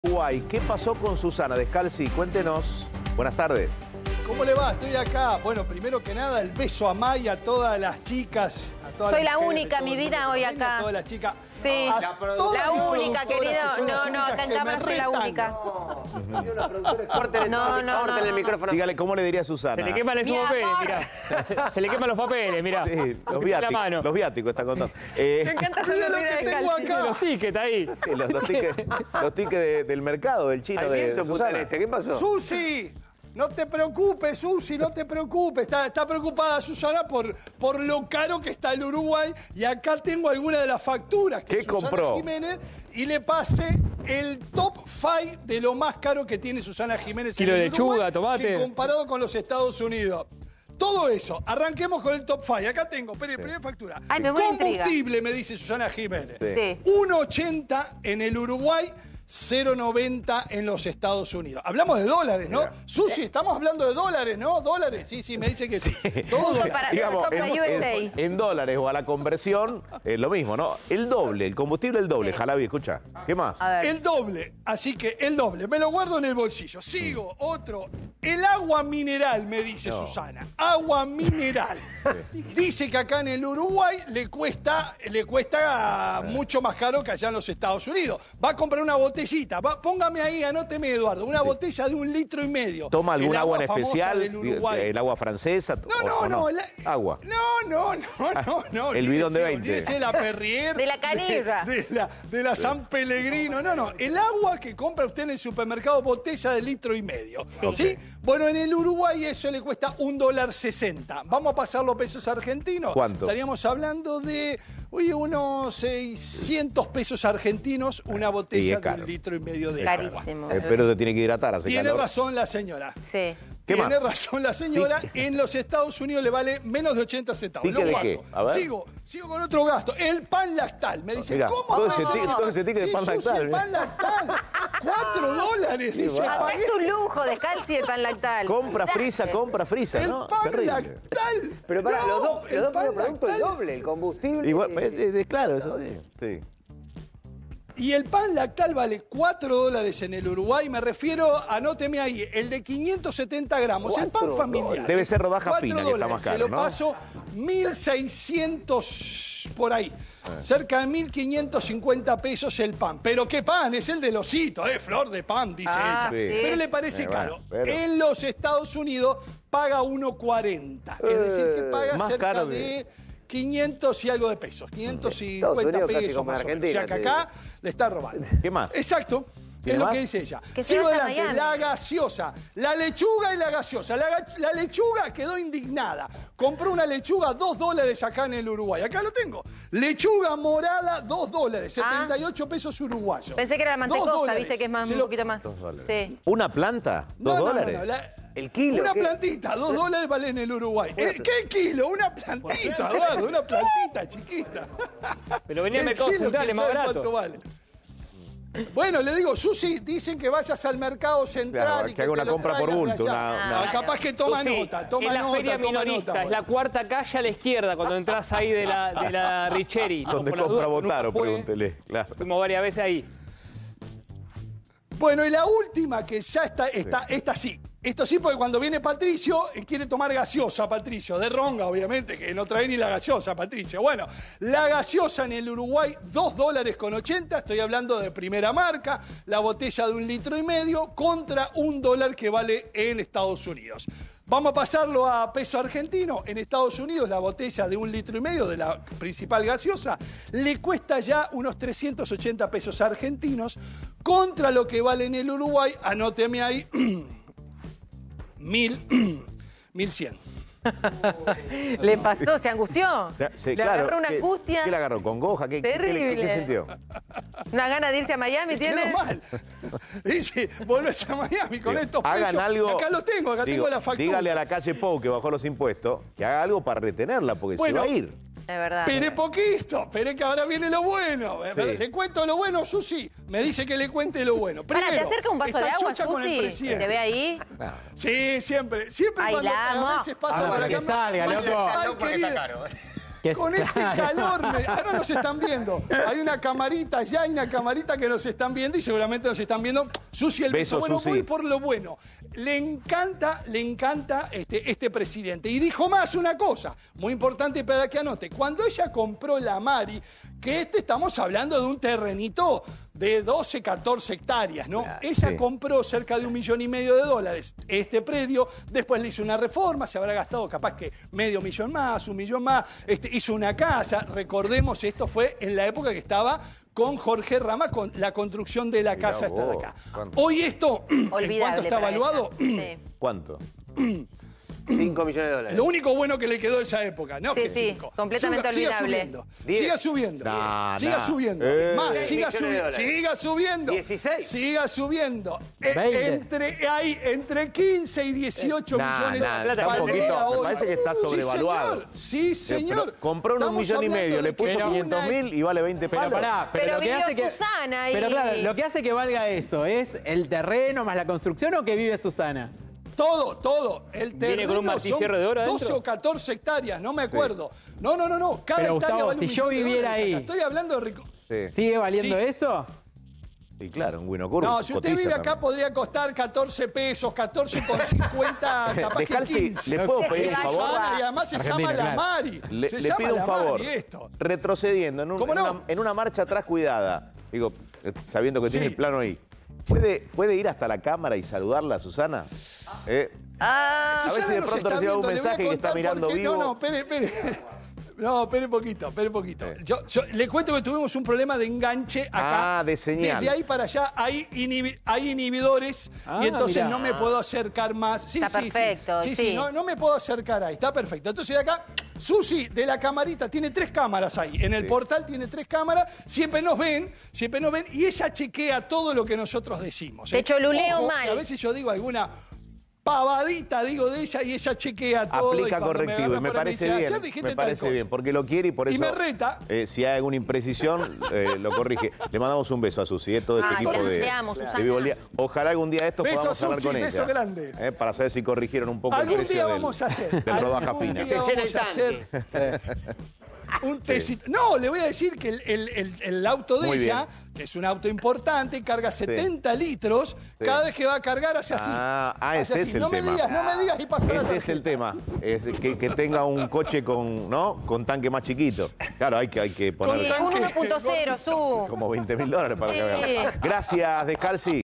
📺 Susana Giménez se queja de los precios de Uruguay: "Gasto más acá que en EE.UU
👉 Si bien la diva de la tv elogió a el presidente Lacalle Pou, también dijo que "Uruguay está muy caro".